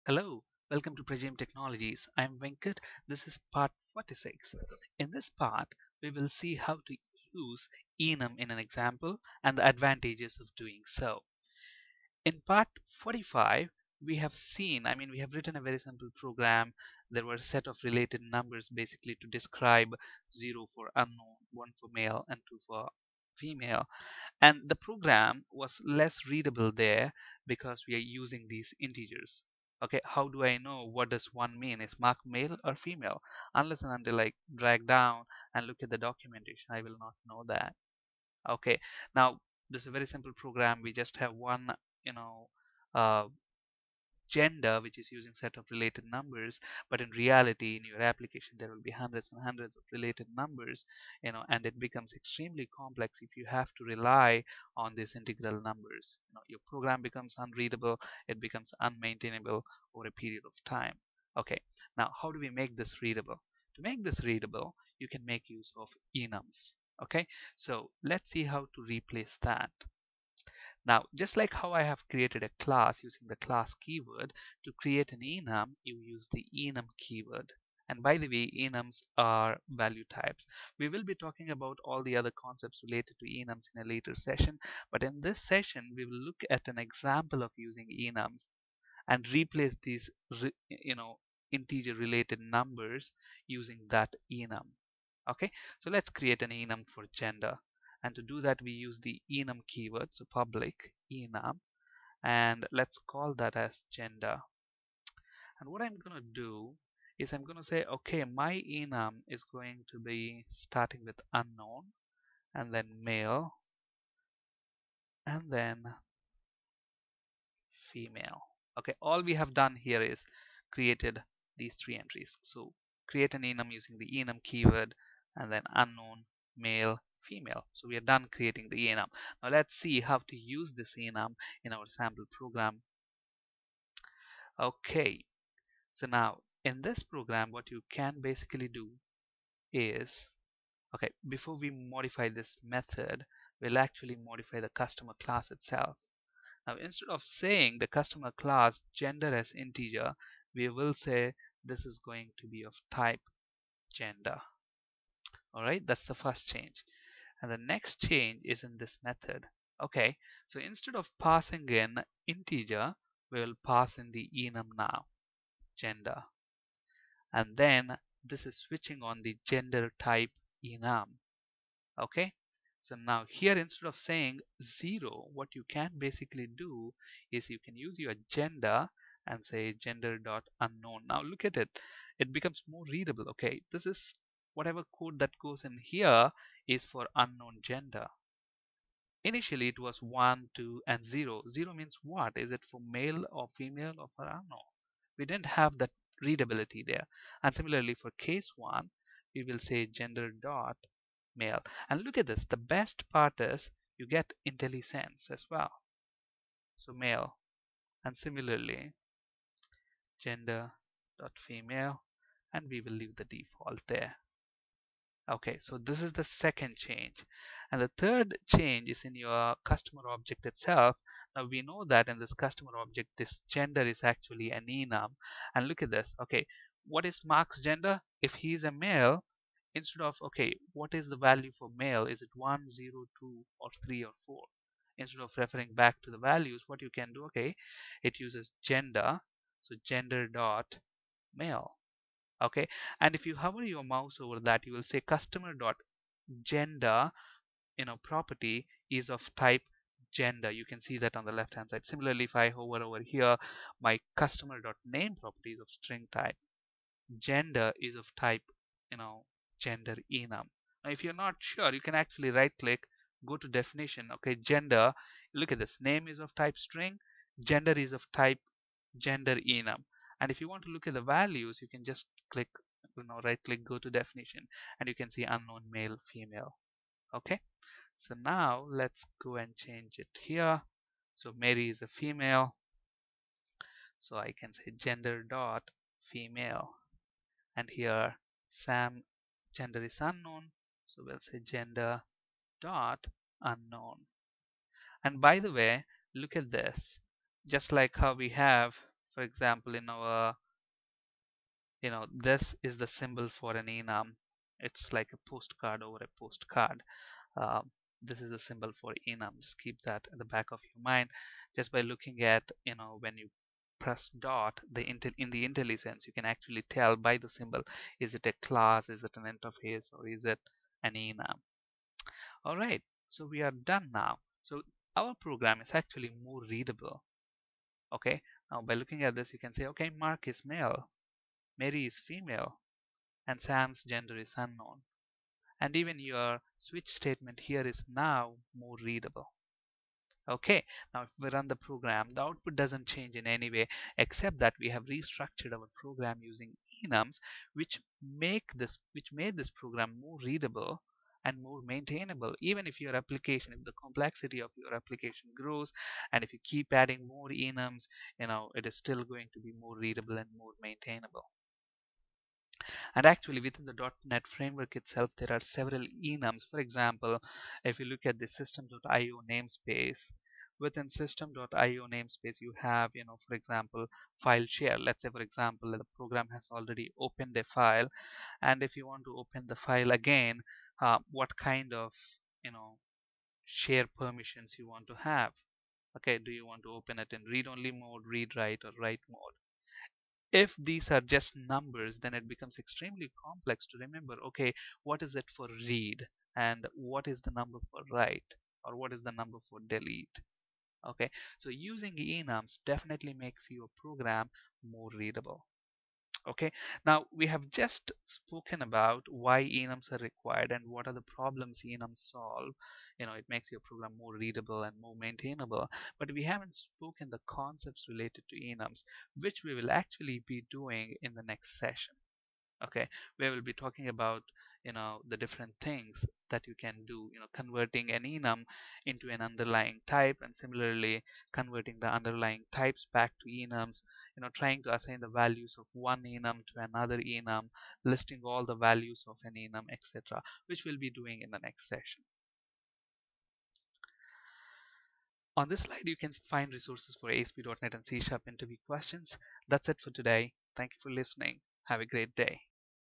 Part 46 C# Tutorial Enums Example
In this session we will learn
1. Using enum in an example
2. The advantages of using enums